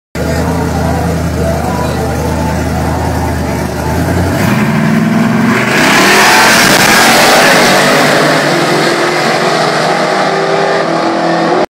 68 Mustang vs nitrous trans am @YelloBelly 3/19/15
Had to re-upload.  YouTube effects made quality worse.  He tree'd the hell put of me but I drove right around him till I broke loose around the 400 ft mark. I had to 100% lift but got back in it FTW.